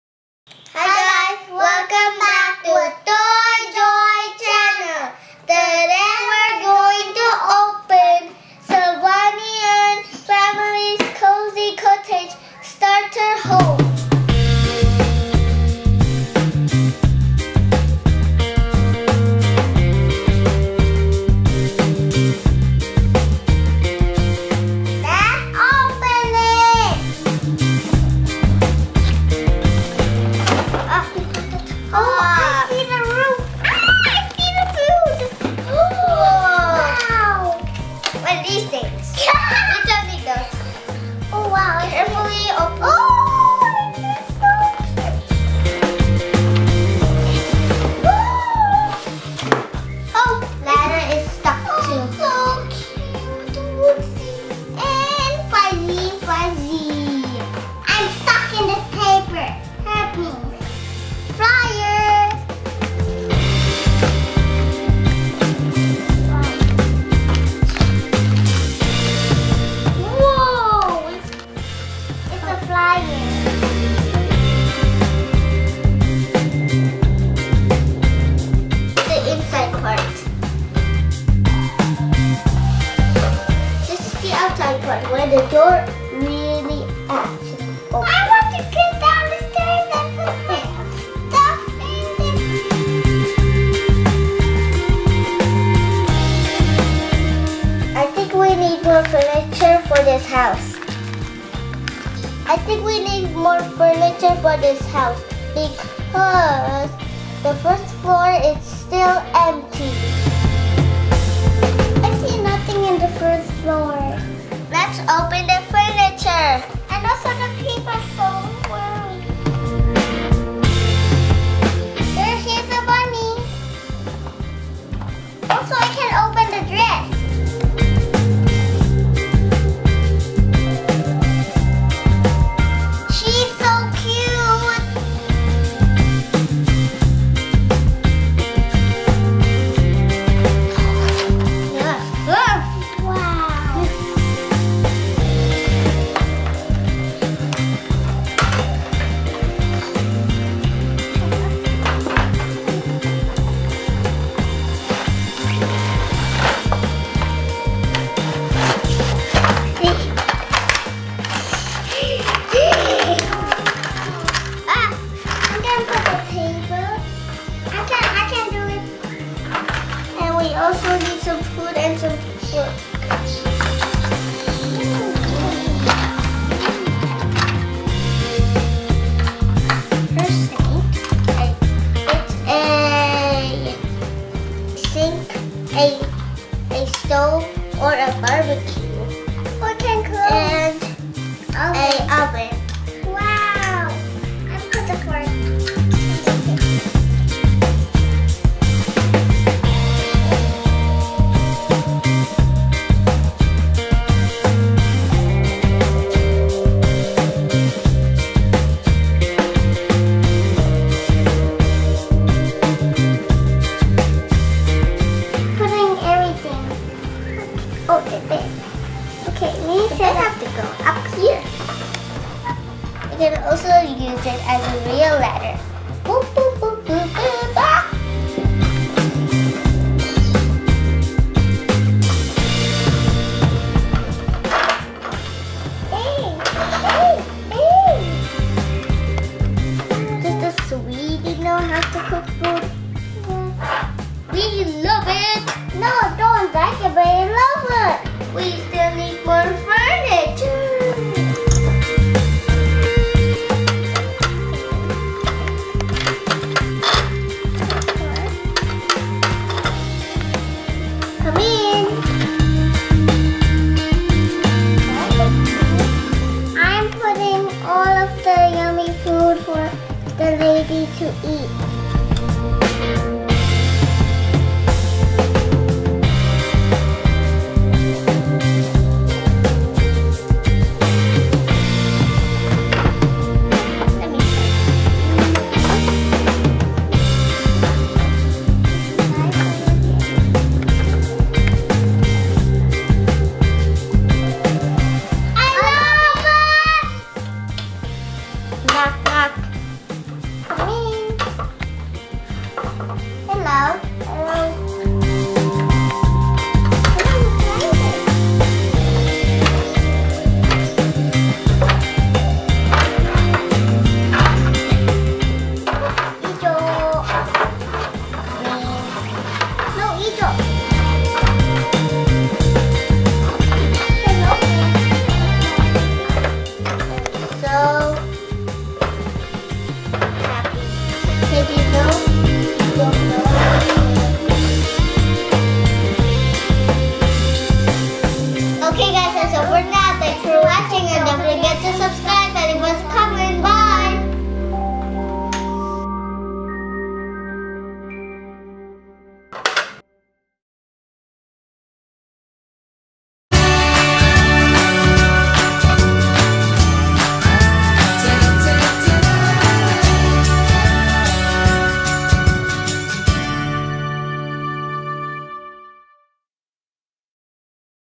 Sylvanian Families, Cosy Cottage Starter Home - Unboxing | Toy Joy Channel
Hi all Sylvanian Families lovers,
Today we're unboxing Sylvanian Families - Cosy Cottage Starter Home toys.
woohoo!